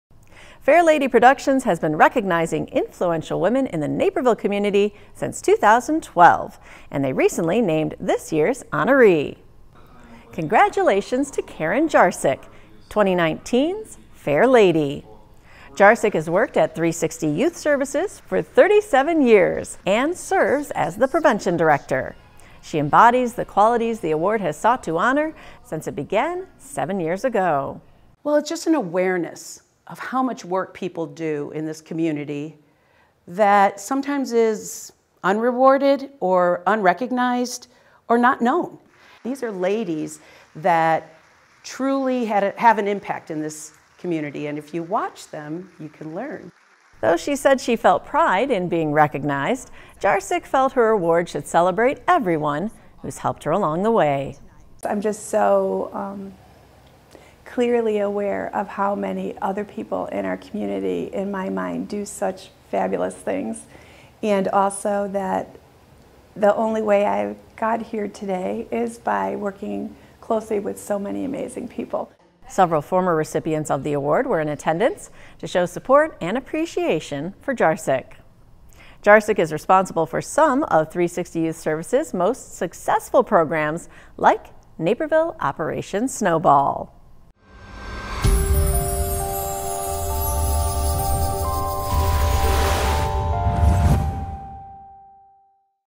2019 Fair Lady Award Presented to Karen Jarczyk
Fair Lady Productions recently named Karen Jarczyk its 2019 Fair Lady Award winner. Jarczyk has worked at 360 Youth Services for 37 years.
Follow us on:
Fair Lady Productions has been recognizing influential women in the Naperville community since 2012, and they recently named Karen Jarczyk this year’s honoree.

Jarczyk has worked at 360 Youth Services for 37 years and serves as the prevention director. She is responsible for some of the organization’s most successful programs, including Naperville Operation Snowball.

Throughout her work, she has embodied the qualities the award has sought to honor since it began seven years ago.

“It’s just an awareness of how much work people do in this community that sometimes is unrewarded or unrecognized or not known. These are ladies that have truly had an impact in this community and if you watch them you can learn,” said Kandiss Hernandez, executive director and founder of Fair Lady Productions.

Though she said she felt pride in being recognized, Jarczyk felt her award should celebrate everyone who has helped her along the way.

“I’m just so clearly aware of how many other people in our community in my mind do such fabulous things,” she said. “And also that the only way I got here today is by working closely with so many amazing people. So I feel like they deserve it too if that makes sense.”

Several former recipients of the award were in attendance to show support and appreciation for Jarczyk.

Naperville News 17’s Casey Krajewski reports.